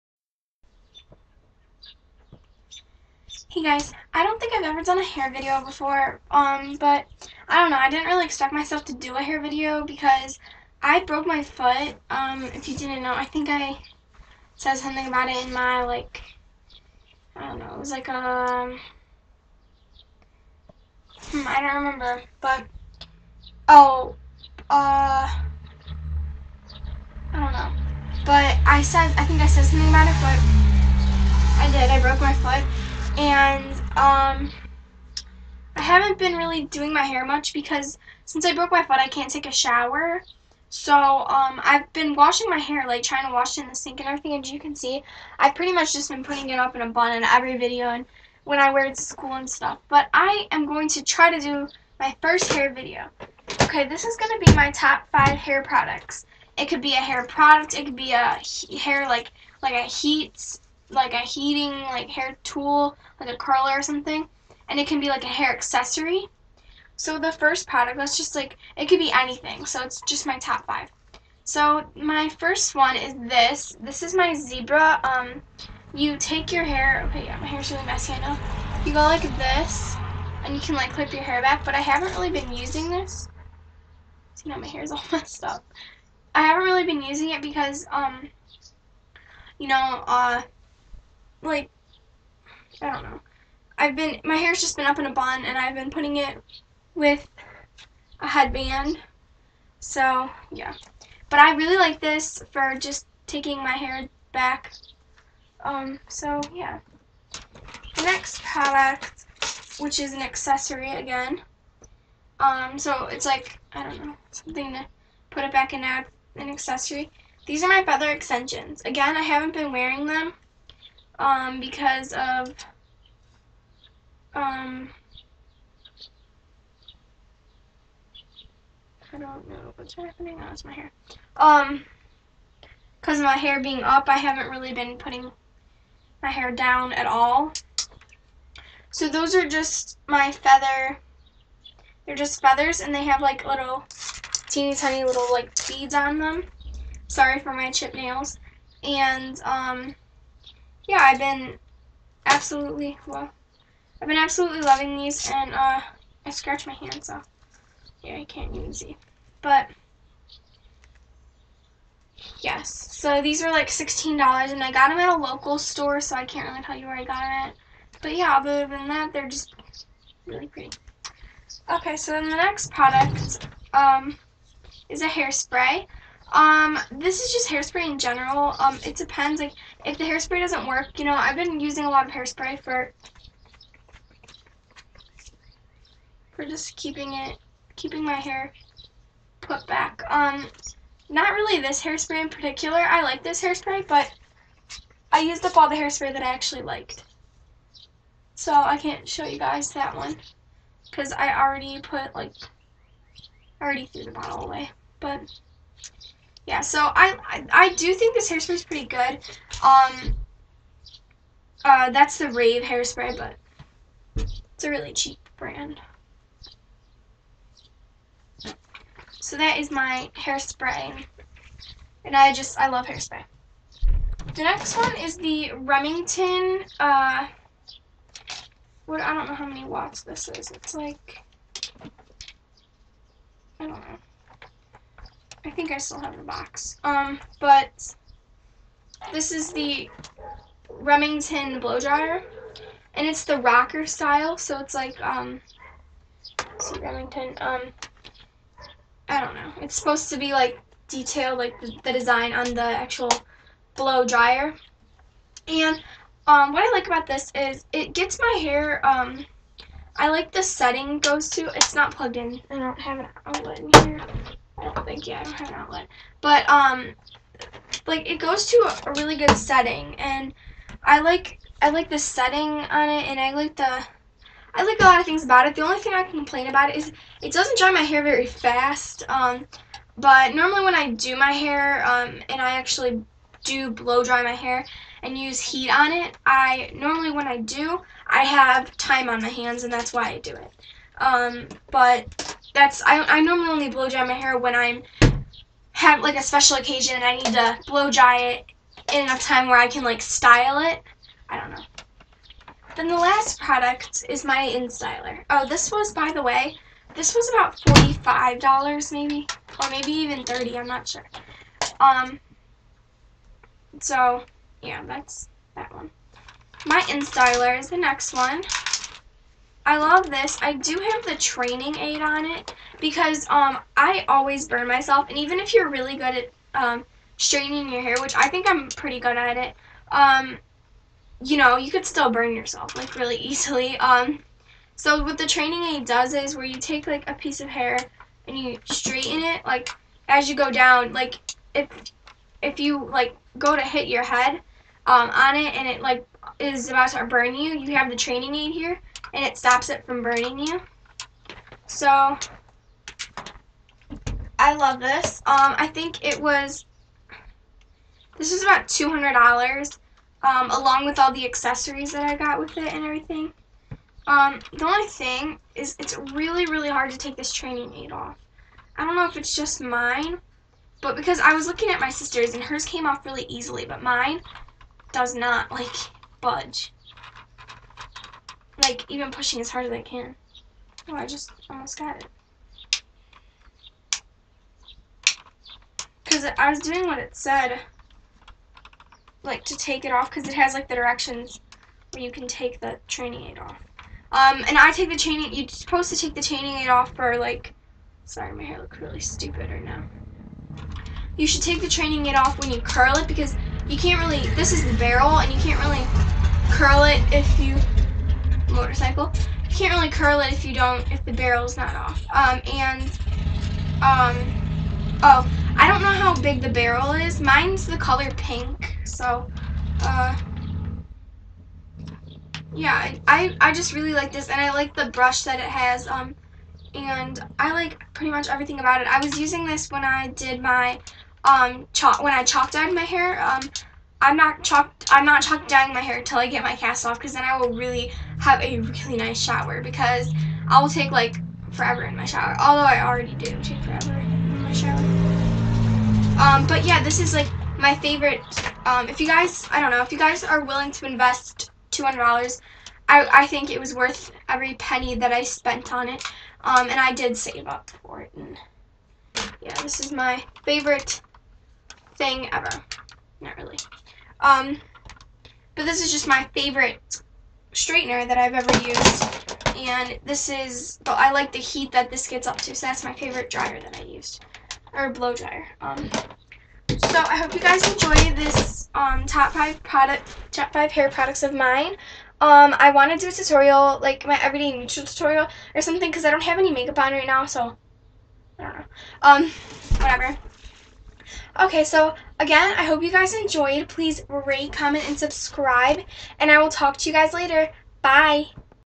Top 5 Hair Products!!
These are the products that I love for my hair!! I can't really do much with my hair currently because I broke my foot! Bye!